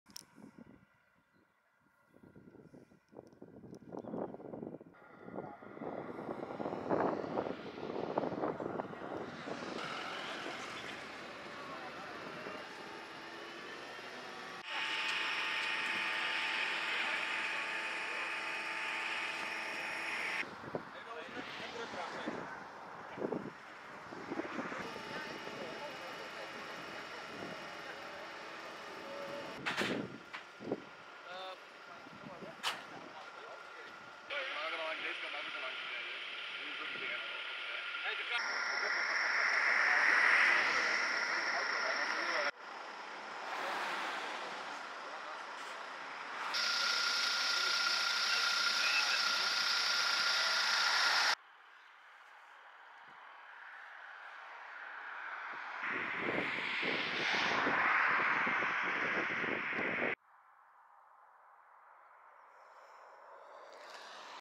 Auto de sloot in bij Peize
Peize - Bij Peize reed een auto de sloot in. De bestuurder kon zelfstandig de auto verlaten. Wel raakte het slachtoffer gewond en werd overgebracht naar het ziekenhuis. De brandweer was ook precent.